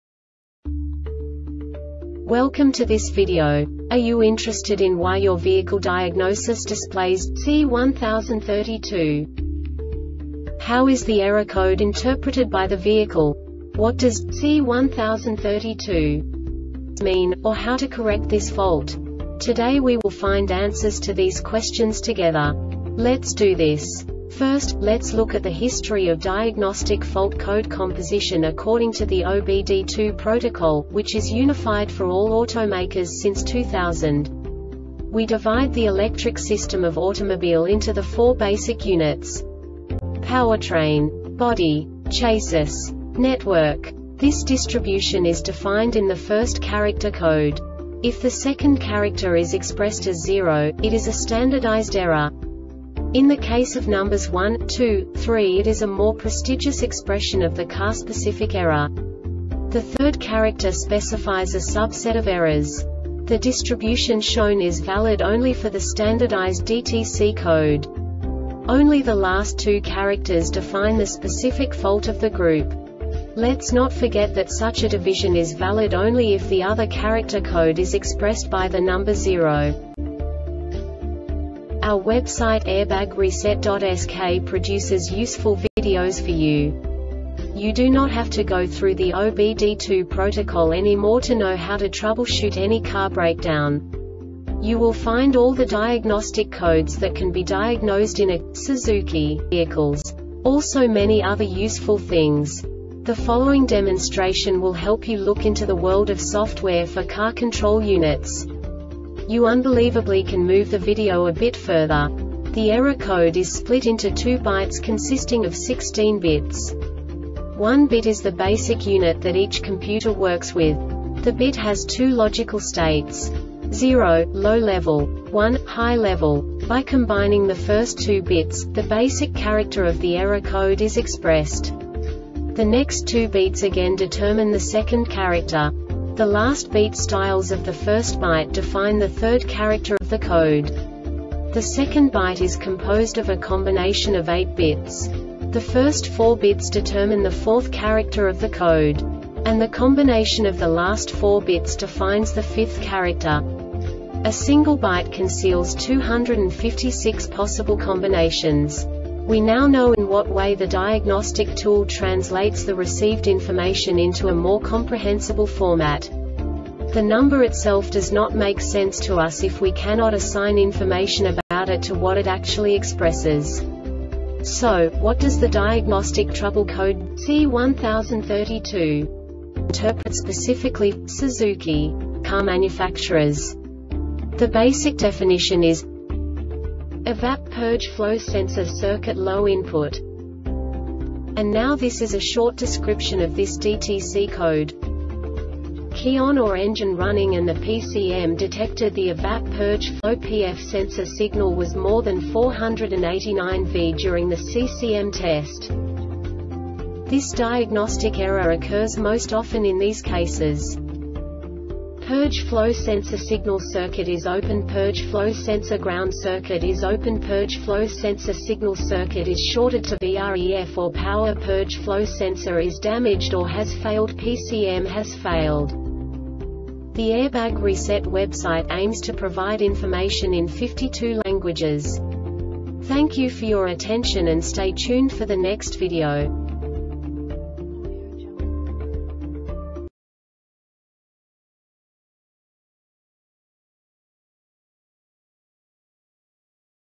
DTC Suzuki C1032 Short Explanation
Contents:
0:21 Basic DTC analysis according to OBD2 protocol standard.
1:48 Insight into programming
2:58 A diagnostically understandable description of the Diagnostic Trouble Code
3:05 Basic error definition

Make:
Suzuki
Code:
C1032
Definition:
EVAP Purge Flow Sensor Circuit Low Input
Description:
Key on or engine running and the PCM detected the EVAP Purge Flow (PF) sensor signal was more than 4.89v during the CCM test.
Cause:
Purge flow sensor signal circuit is openPurge flow sensor ground circuit is openPurge flow sensor signal circuit is shorted to VREF or powerPurge flow sensor is damaged or has failedPCM has failed